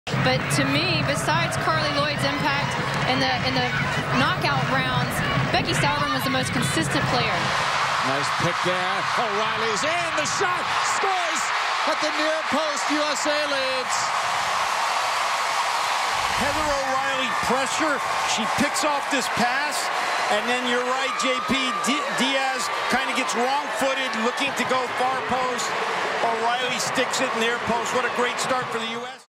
WNT vs. Costa Rica: Heather O'Reilly Goal - Aug. 16, 2015
Heather O'Reilly put the USA on the board early in the opening match of the 2015 USWNT Victory Tour.

U.S. Soccer